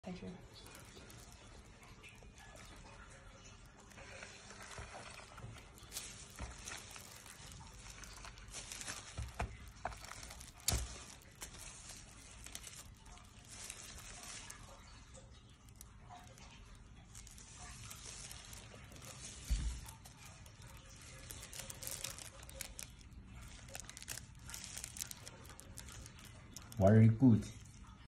Sanitary sheath Chemise sanitaire
Sanitary protection in the form of a bag!  Unroll a bag, slip the sheath and gun inside, then advance the sheath and gun once inside the cow. The sheath will keep your sheath and gun clean and your procedure sanitary.
The sanitary chemise plastic oversleeve is pulled over the transferor insemination rod immediately prior to entering the cow or heifer. Upon contact with the cervix, the sleeve is pulled back forcing the clean rod through the sleeve.
 If you need OEM Chemises Sanitaires, pls send email to us.